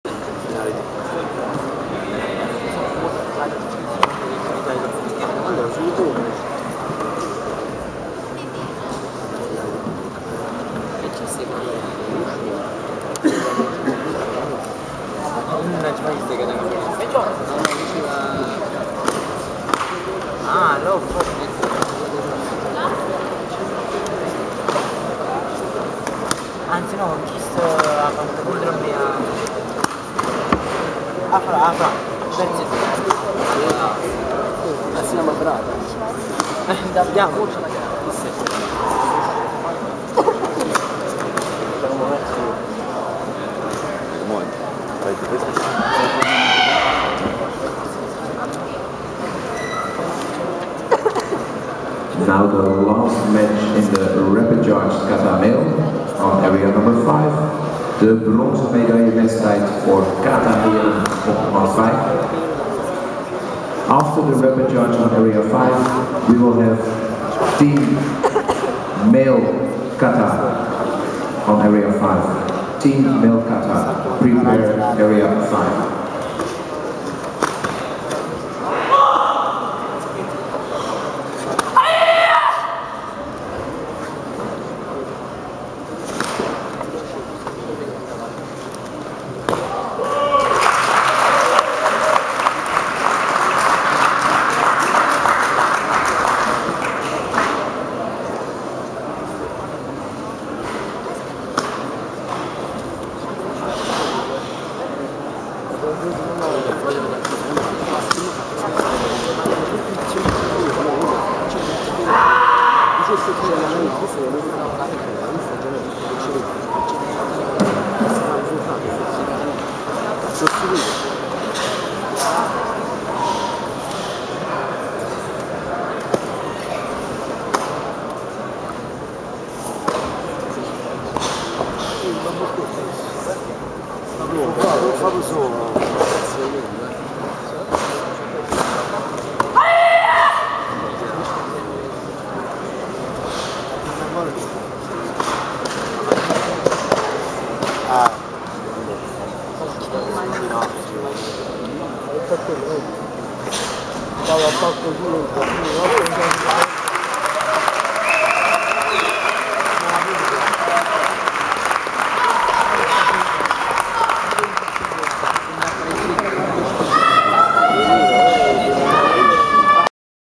Rika Usami - Chatanyara Kushanku
Kata Chatanyara Kushanku by Rika Usami at Dutch Open karate 2012.